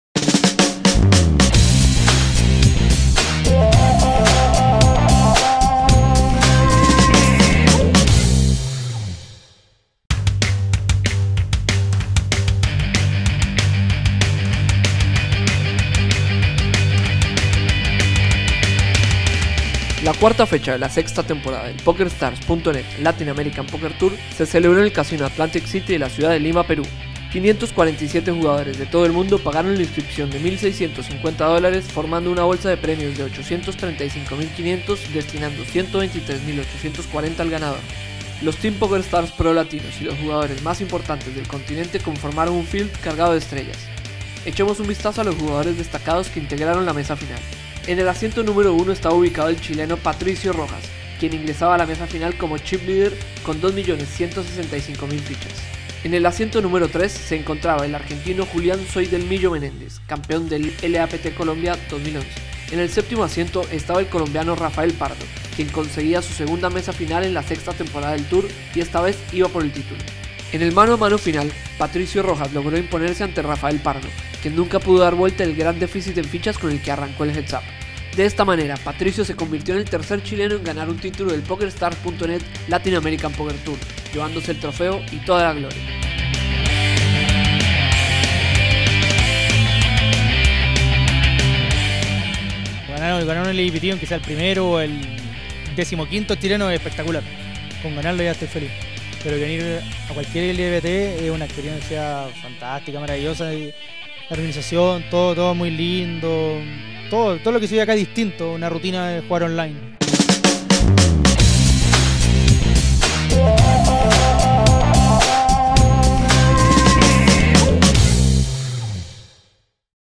LAPT Perú 2013 - Resumen
Todo el resumen de lo que nos dejó el PokerStars.net Latin America Poker Tour Perú 2013